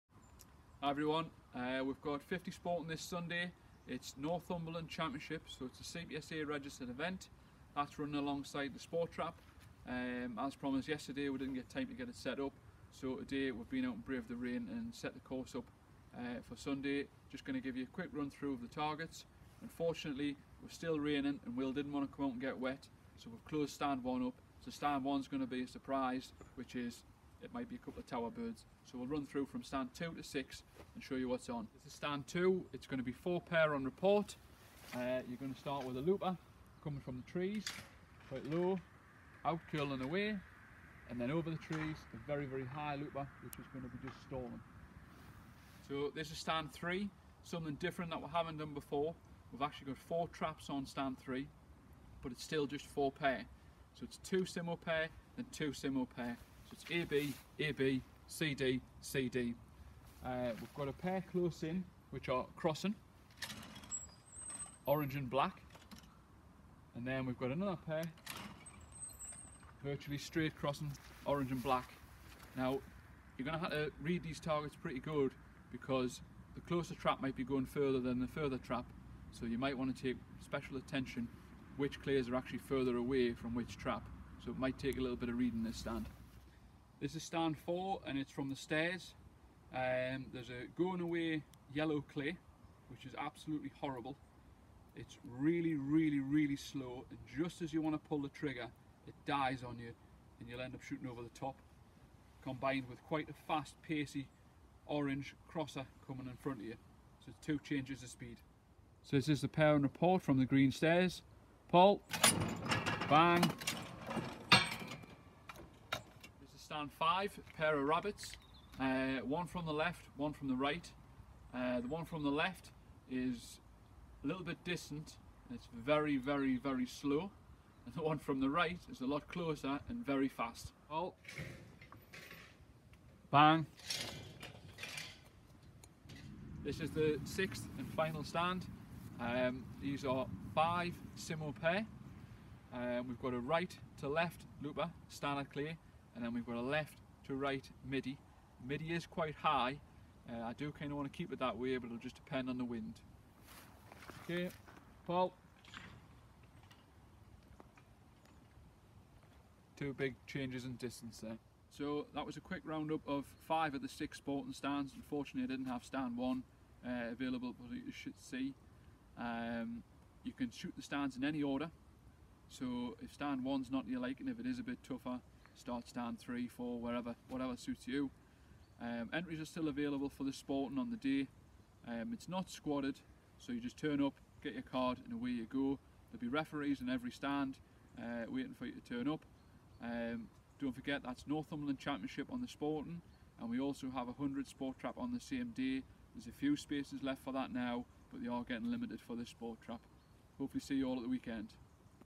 50 Sporting Northumberland Championship target preview
A look at the upcoming targets for our 50 Sporting Northumberland Championship on Sunday 20th October 2019.